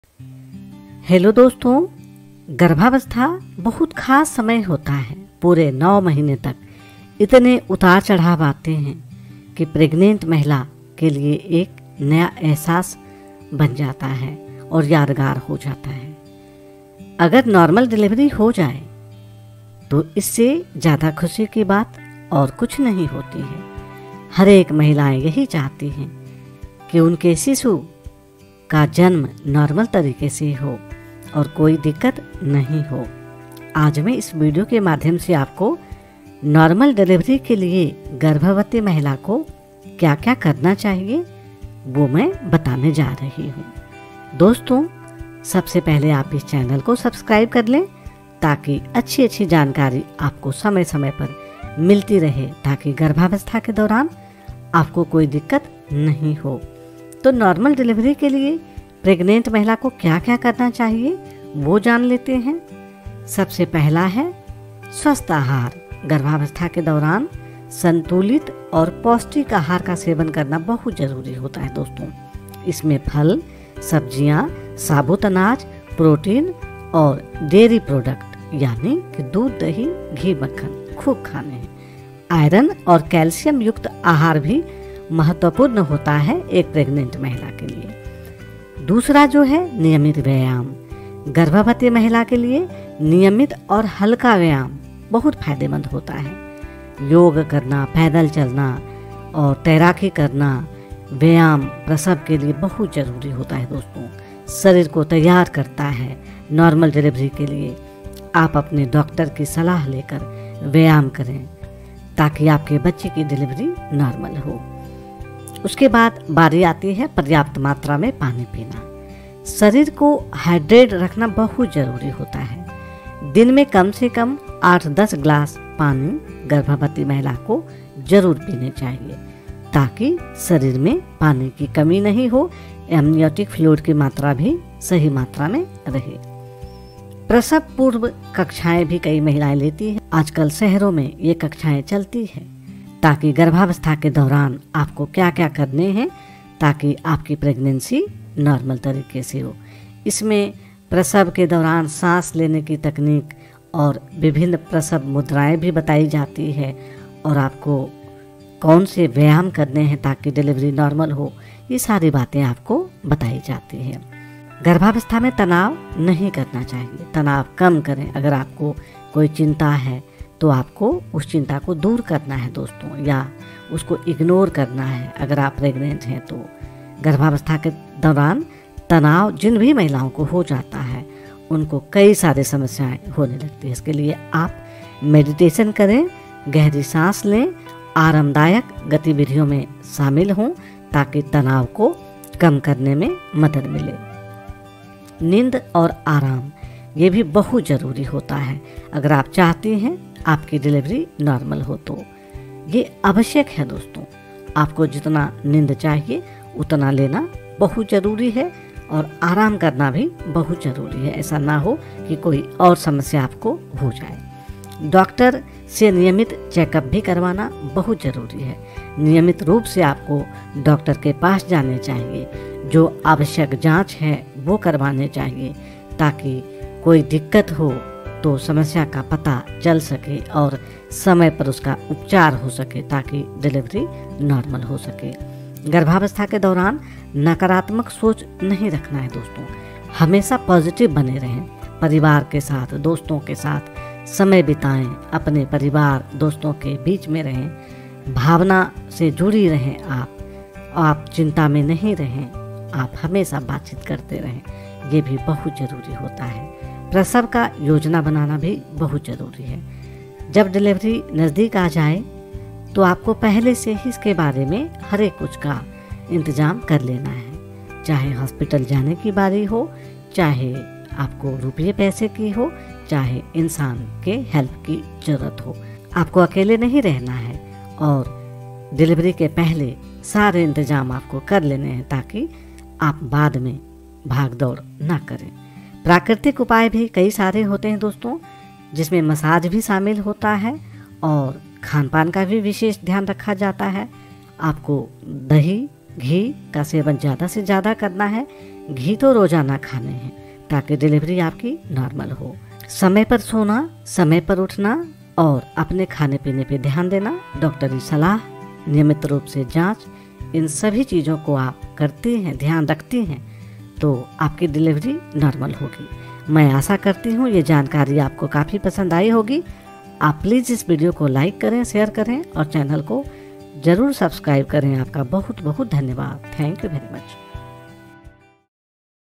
Normal Delivery Tips: What Should You Do for a Smooth Delivery
Preparing for a normal delivery? In this video, we discuss essential tips and practices that can help you achieve a safe and smooth delivery. From staying active with the right exercises to maintaining a balanced diet, managing stress, and practicing relaxation techniques, these expert-recommended steps will prepare you for labor. Our insights are specially tailored with a Global Indian Pregnancy Care approach to support moms-to-be. Watch now to empower your pregnancy journey with confidence!